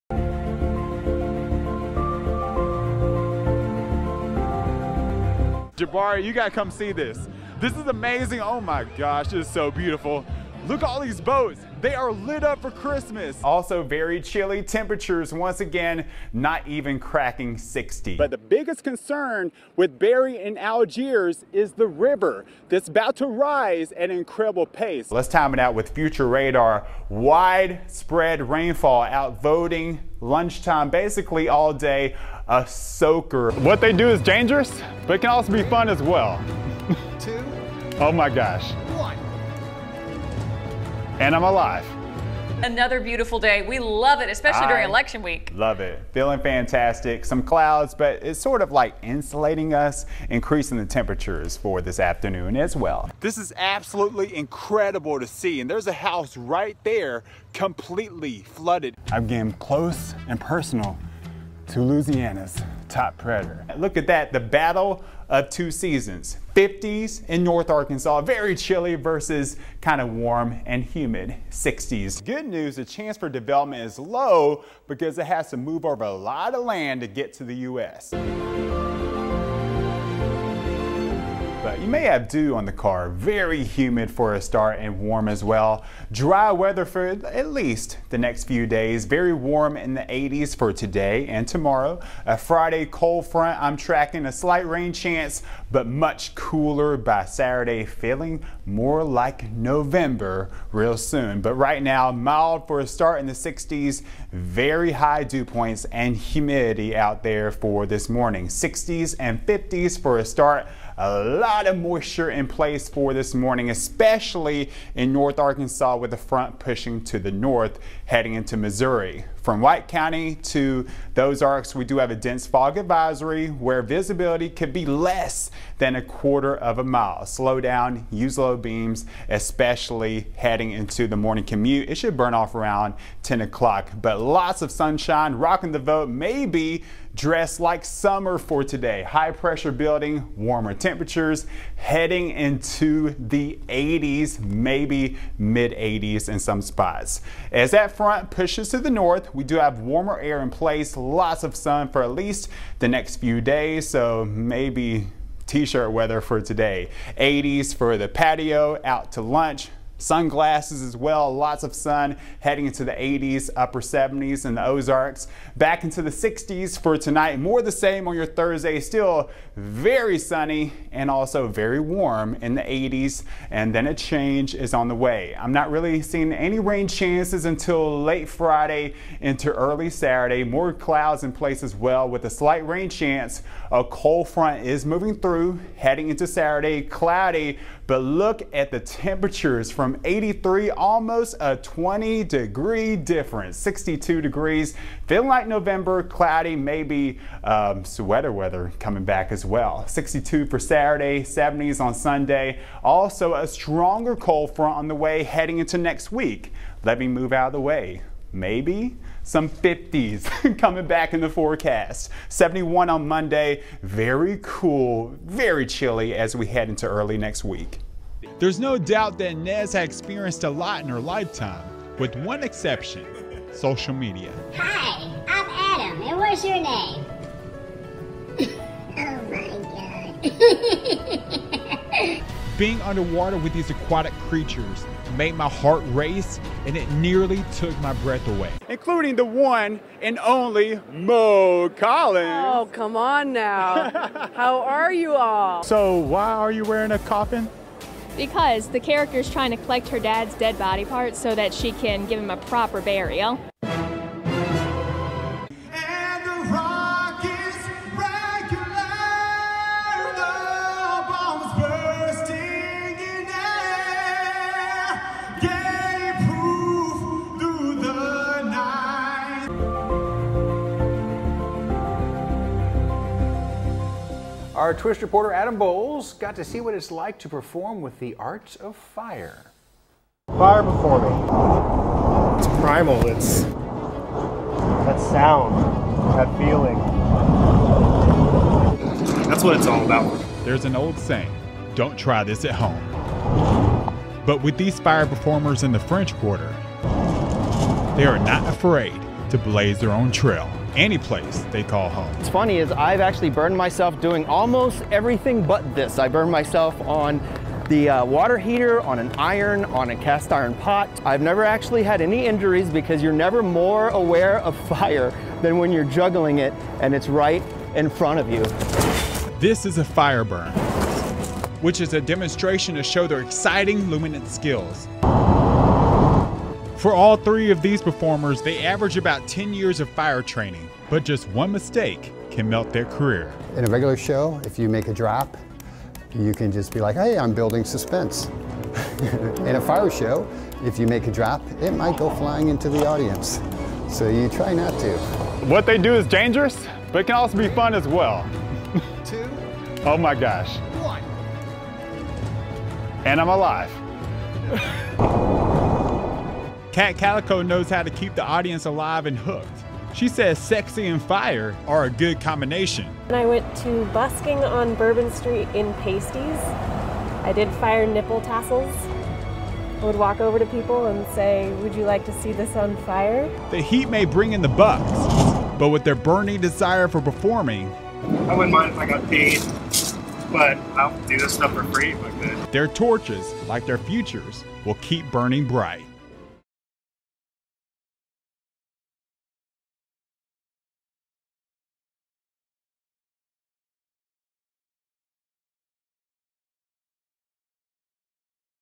Adam Bowles Weather/Reporter reel 2022
This reel first begins with a montage of by best work :05
Then it leads into my full forecast 1:22
After that, I have small clips that show more of my personality and creative side.
Ending with my MMJ package 4:32